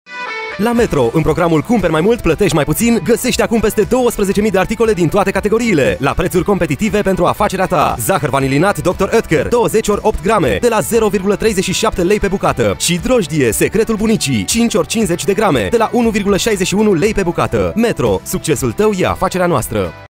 METRO - Cumperi mai mult, platesti mai putin - Zahăr vanilinat Dr. Oetker & drojdie Secretul Bunicii
La METRO, în programul Cumperi Mai Mult plătești mai puțin,
găsești acum peste 12.000 de articole din toate categoriile, la prețuri competitive pentru afacerea ta!
Zahăr vanilinat DR OETKER, 20x8 grame, de la 0,37 lei pe bucată, și drojdie Secretul Bunicii, 5x50 g, de la 1.61 lei pe bucată.
Metro. Succesul tău e afacerea noastră.